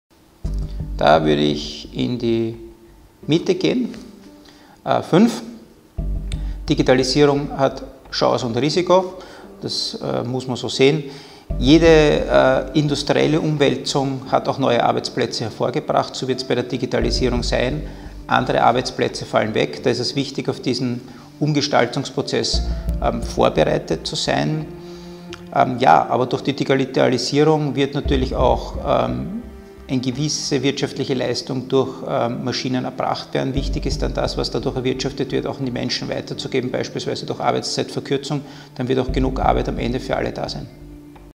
10 parlament2017 | Albert Steinhauser (GRÜNE) | Digitalisierung
Albert Steinhauser (Grüner Klubobmann) im neuwal Barometer parlament2017.
19 Positionen zur politischen Lage.
neuwal Barometer
Wie gut kennst Du Kandidaten?
Und wer ist Dein Kandidat?